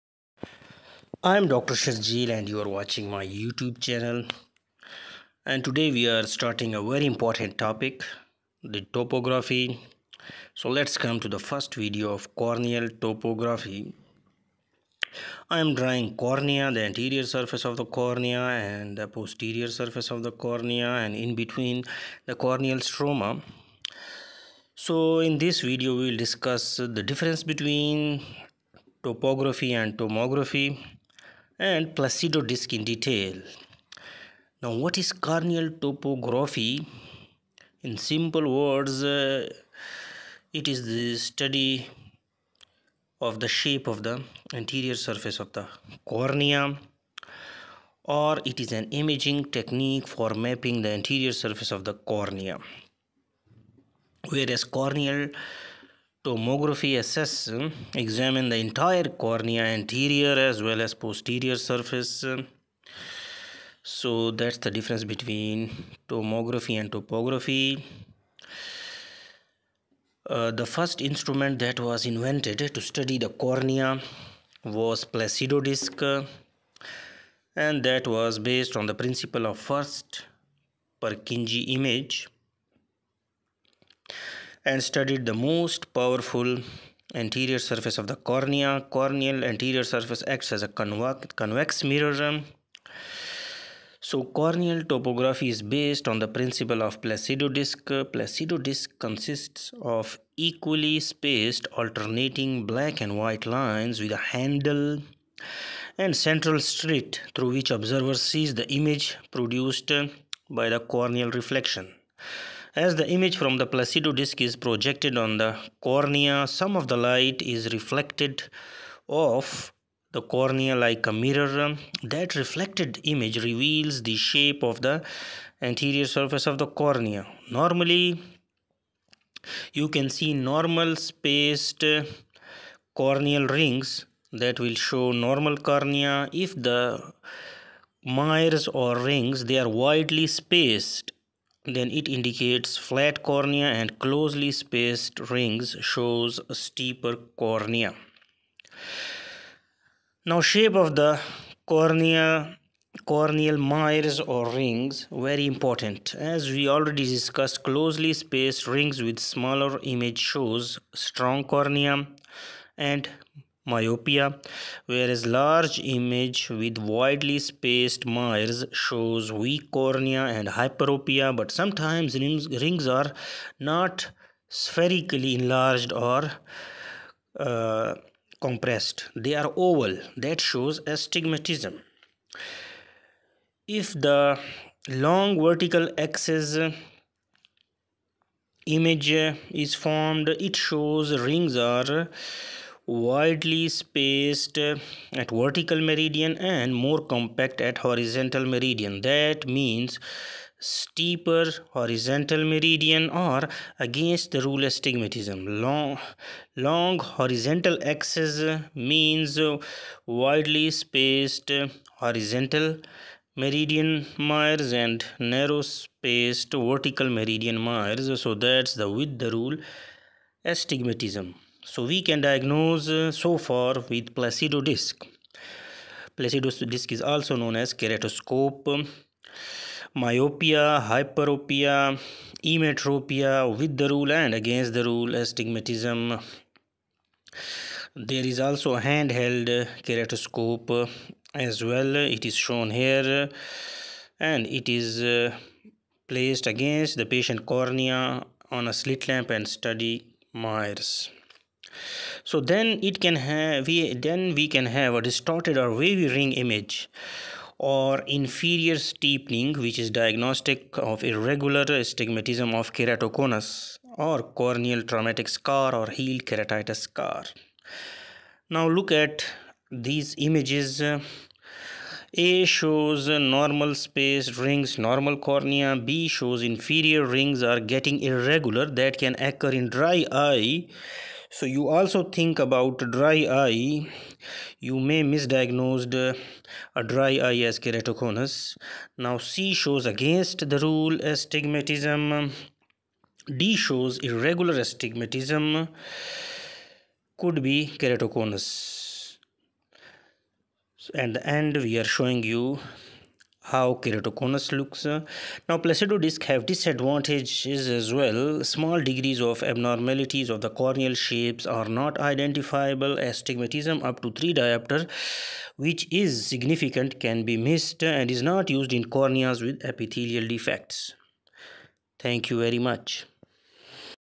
Placido disc CT series 1
In this first video of corneal topography we will discuss difference between topography and tomography and placido disc in detail